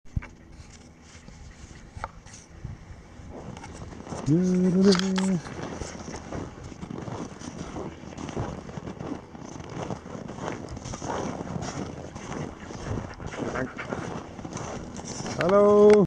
Dogsledding
Mushing with Trail Breaker Kennel in Fairbanks